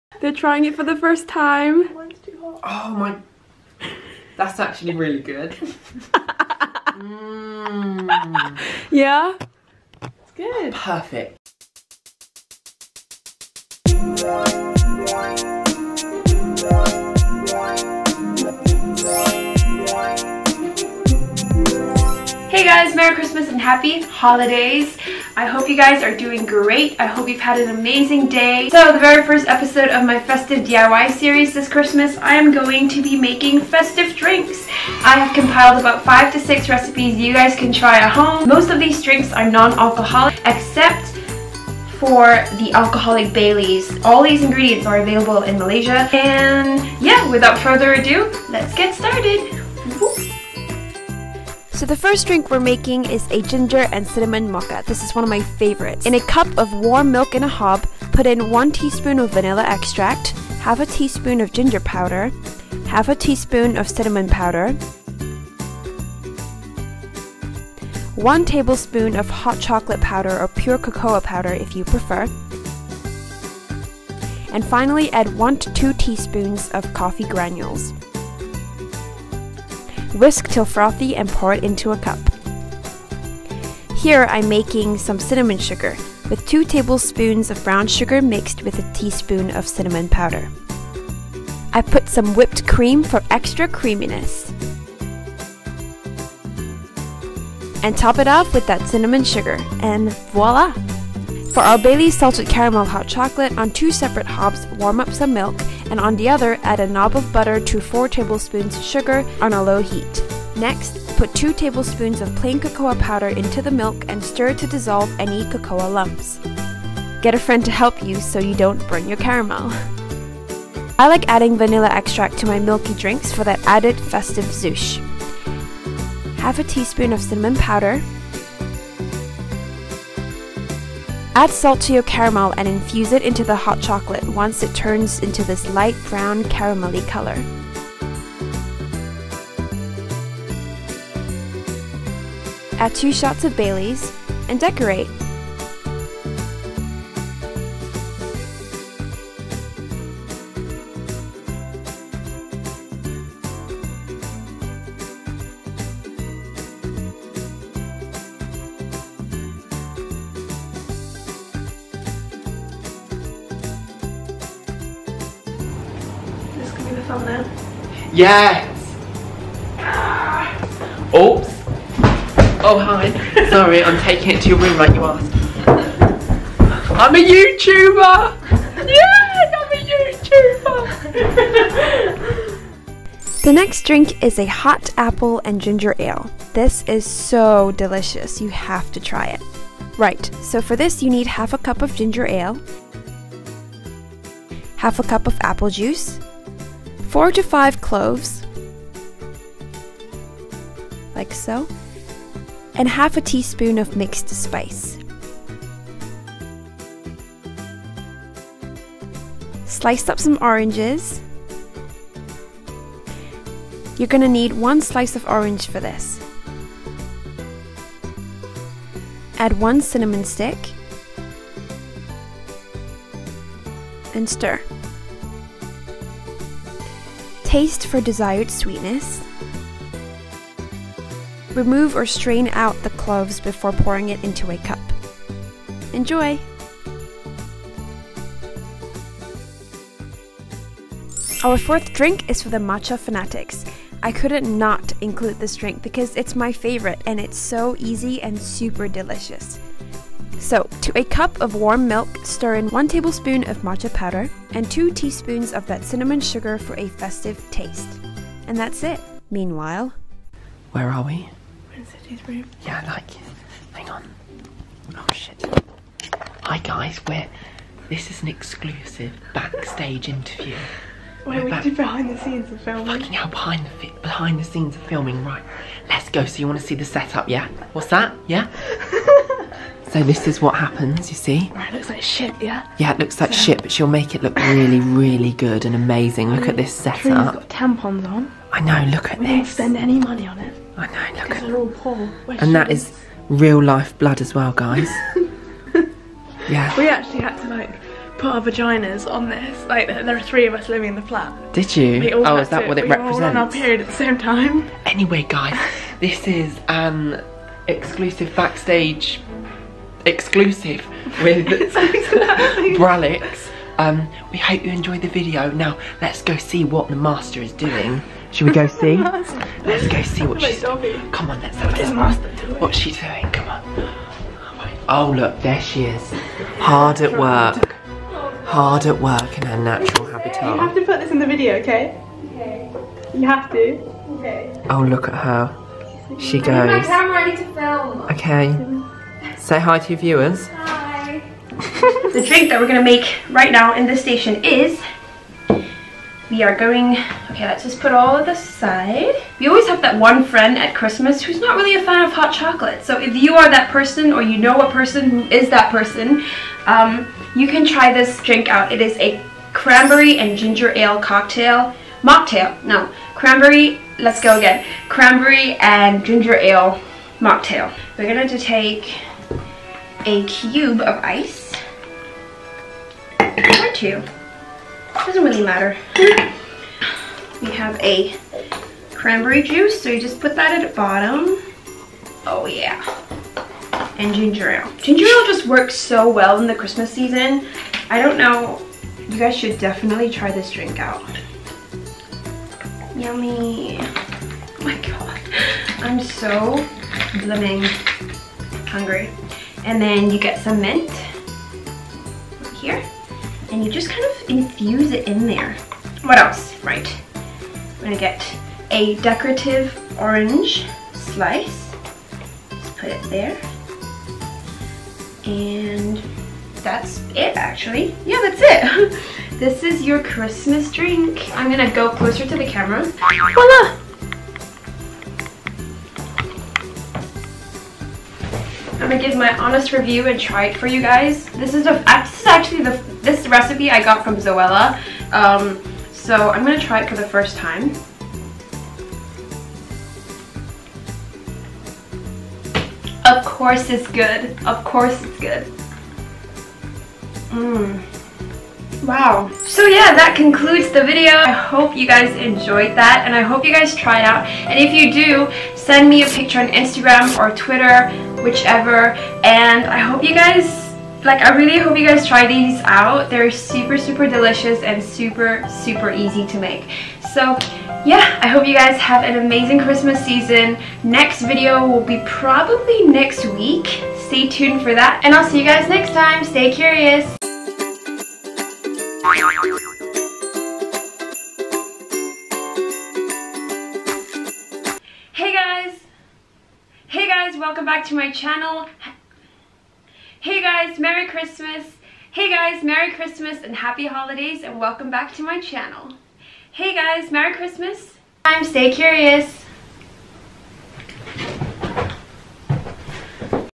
5 DIY CHEAP & EASY festive drinks you MUST TRY!| VLOGMAS EP.1
I recreated and compiled 5 SUPER EASY & INEXPENSIVE festive drinks recipes! Hope you try it out! You won't regret it!